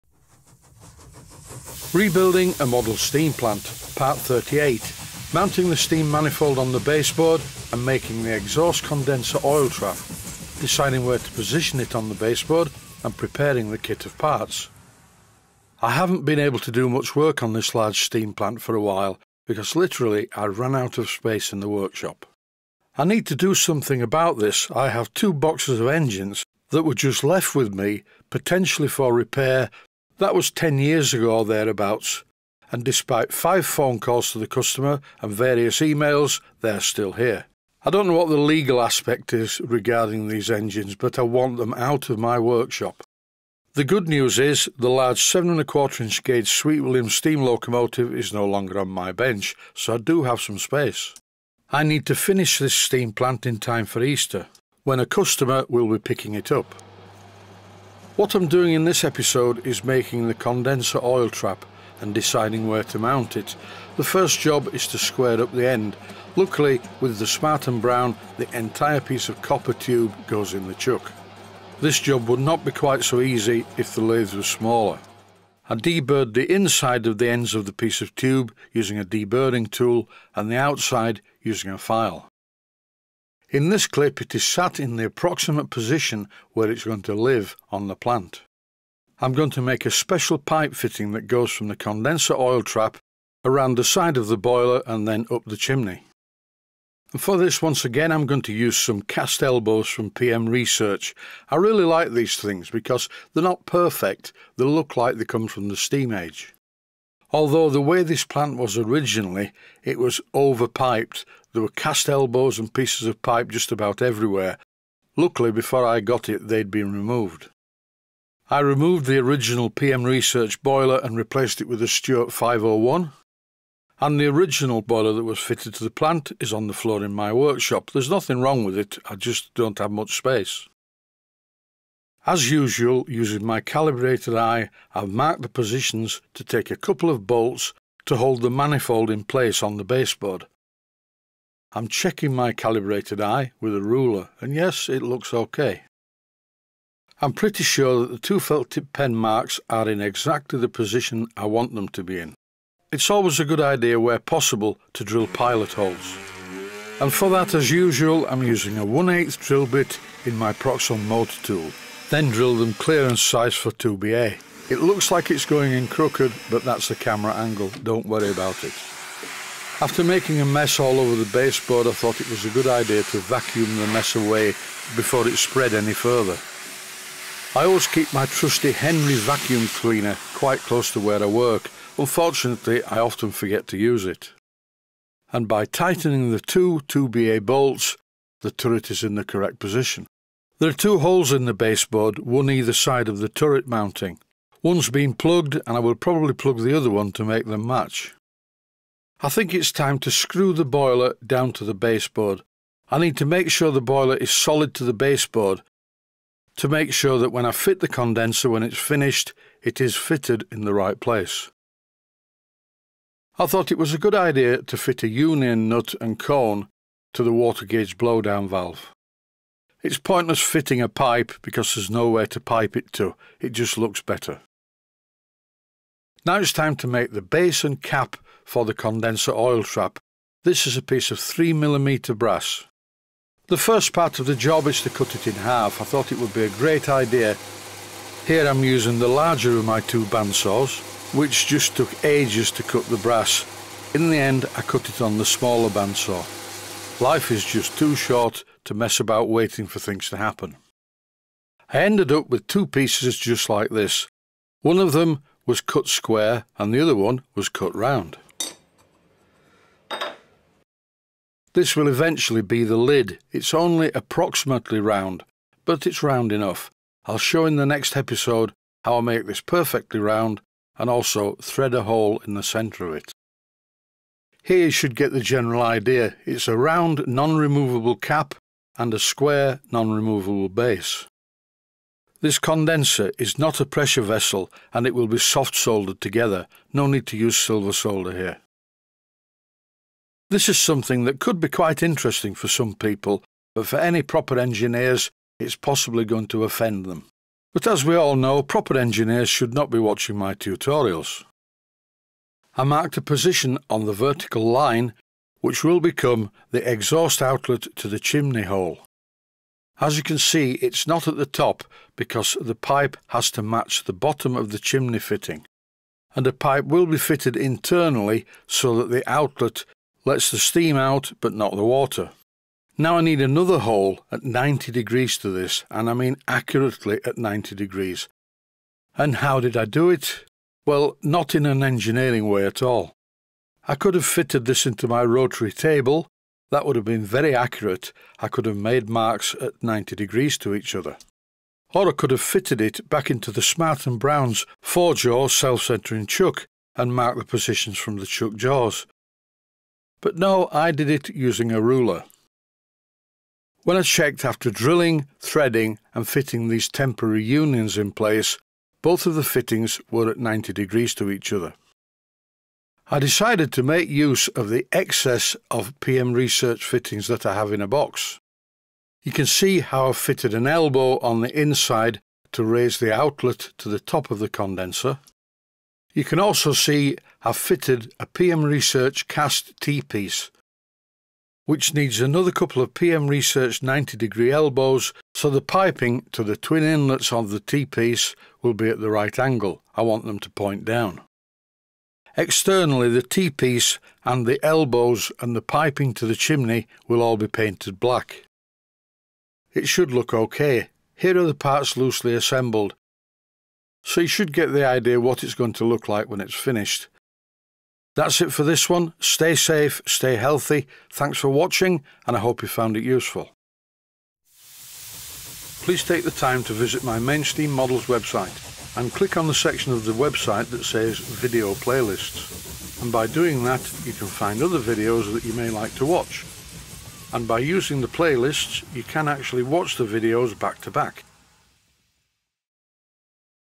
REBUILDING A MODEL STEAM PLANT - PART #38
Rebuilding A Model Steam Plant - Part #38. Mounting the Steam Manifold on the Baseboard and making the Exhaust Condenser/Oil Trap. Deciding where to position it on the Baseboard and preparing the kit of Parts.